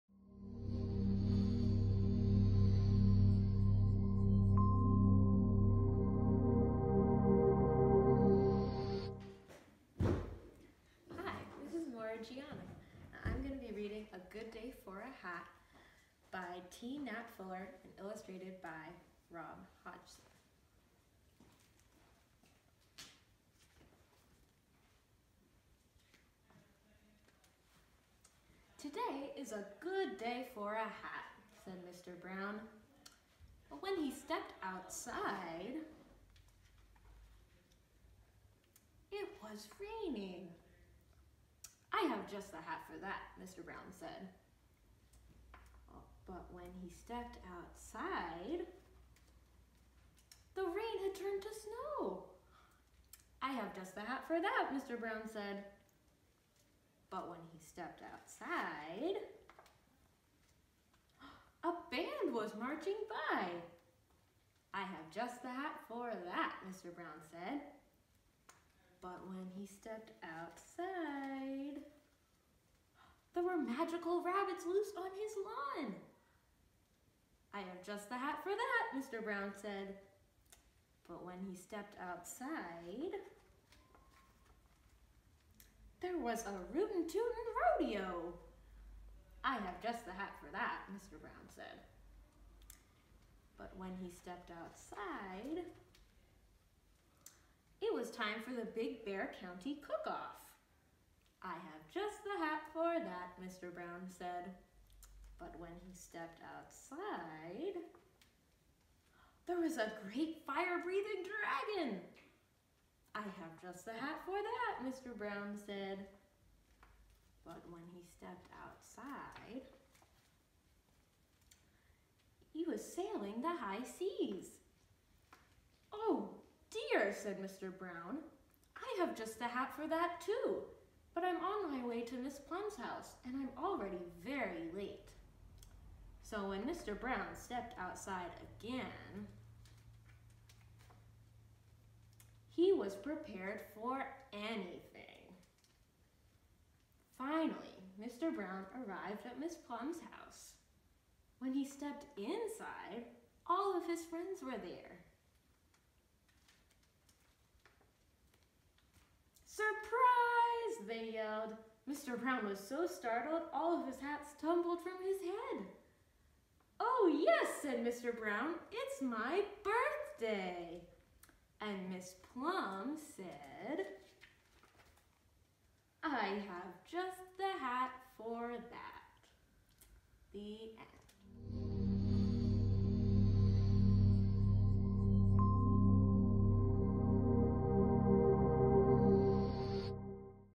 "A Good Day For A Hat" by T. Nat Fuller read aloud by Morah Gianna
This video is about "A Good Day For A Hat" by T. Nat Fuller read aloud by Morah Gianna